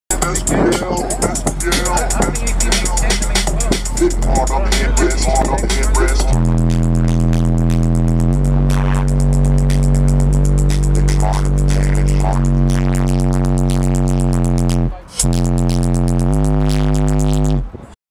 2 10" American Bass XFL - StreetSPL - JC's
Just a little video of 2 10" American Bass XFL. July 7th StreetSPL here in San Antonio at JC's Tint & Alarm Audio hope to see yall there!